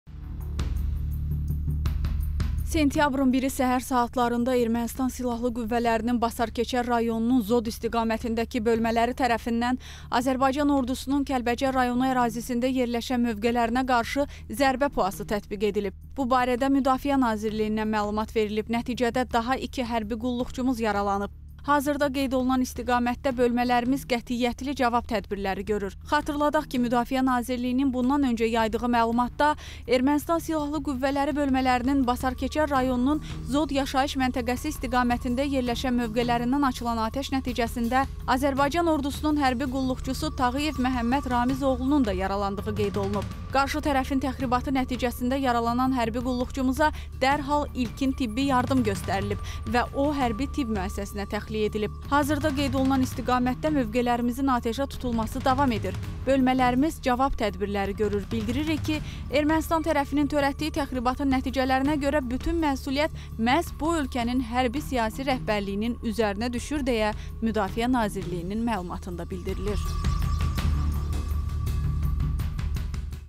Sərhəddə atışma davam edir, 3 hərbçimiz yaralandı
Sentyabrın 1-i səhər saatlarında Ermənistan Silahlı Qüvvələrinin Basarkeçər rayonunun Zod istiqamətindəki bölmələri tərəfindən Azərbaycan Ordusunun Kəlbəcər rayonu ərazisində yerləşən mövqelərinə qarşı zərbə PUA-sı tətbiq edilib.
Bu barədə Müdafiə Nazirliyindən məlumat verilib. Nəticədə daha iki hərbi qulluqçumuz yaralanıb.
Hazırda qeyd olunan istiqamətdə bölmələrimiz qətiyyətli cavab tədbirləri görür.